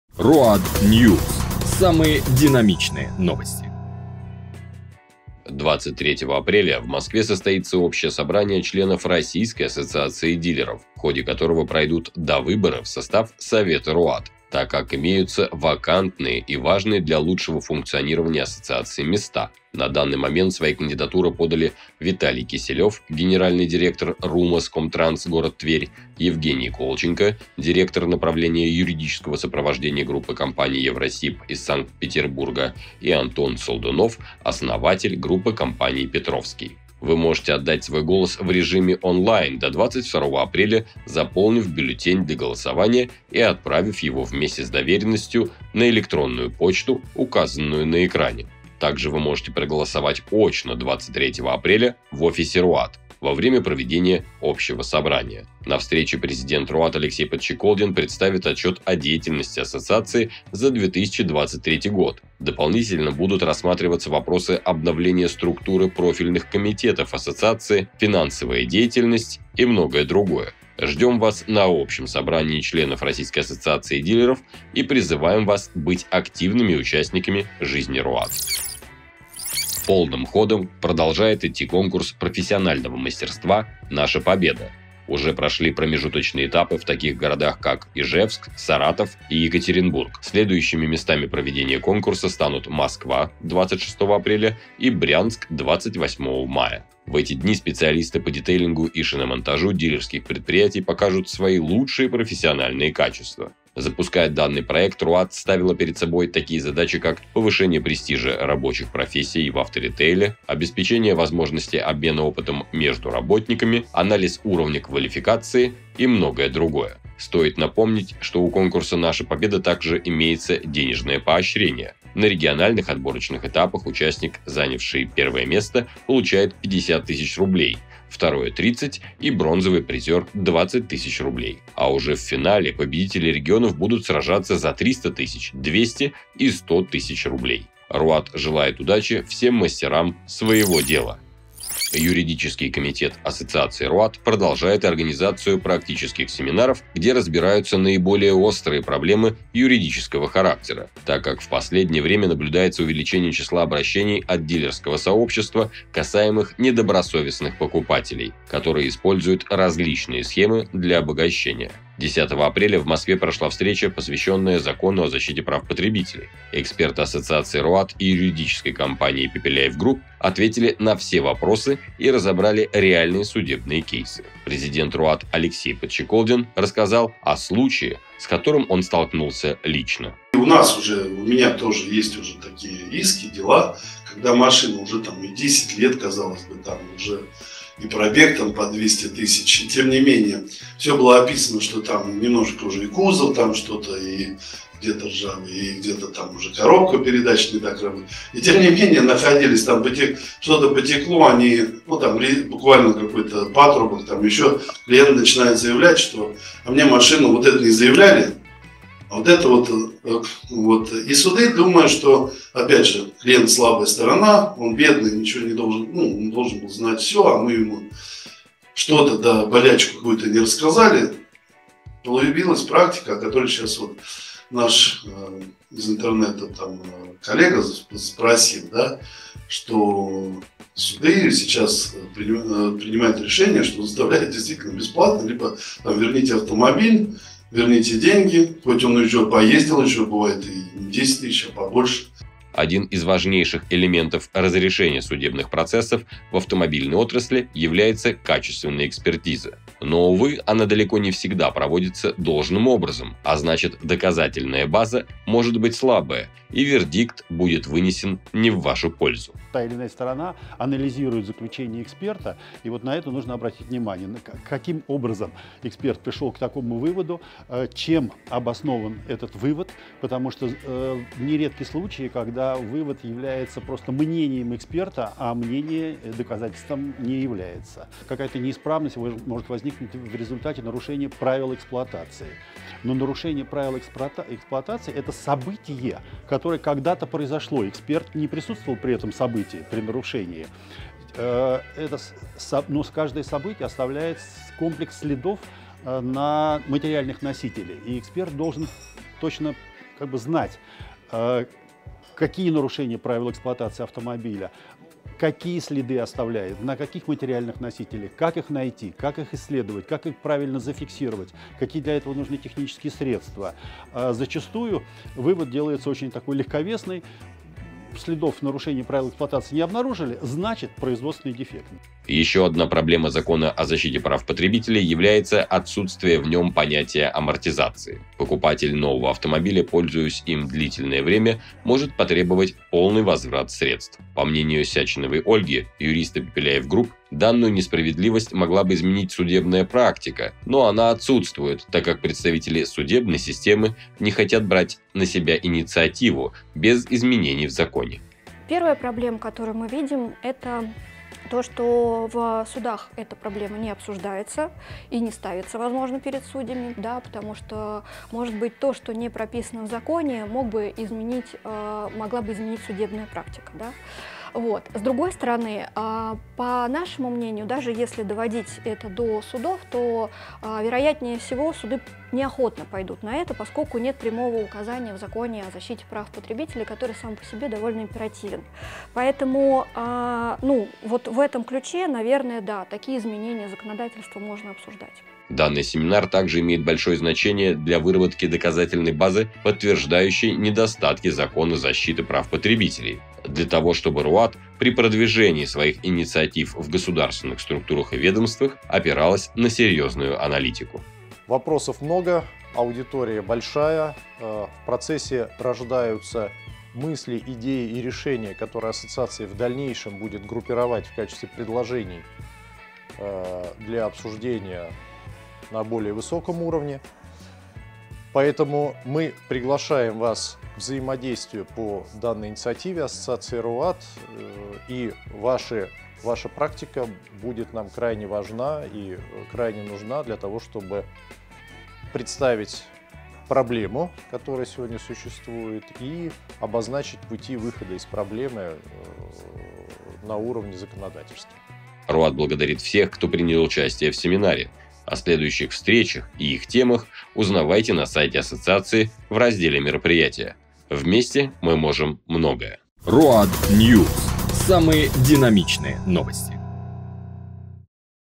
Какие дилеры войдут в "большой" Совет РОАД? NEW! Новые номинации Конкурса НАШАПОБЕДА2024! ЗПП
В сегодняшнем выпуске РОАД-ТВ:

00. 01 Кто будет представлять авторитейл в Совете РОАД? Вопросы для обсуждения на Общем собрании членов РОАД.
01.20 НАШАПОБЕДА2024 шагает по стране! Наконец: НОВЫЕ НОМИНАЦИИ В СОРЕВНОВАНИИ! Подробности в сюжете, а подать заявку на 4-й и 5-й этапы можно на сайте РОАД

02.20 Будут вноситься изменения в ЗПП? Репортаж с семинара РОАД по ЗПП. Комментарии экспертов по основным проблемам: на что опираться при вынесении своих предложений и чего ожидать?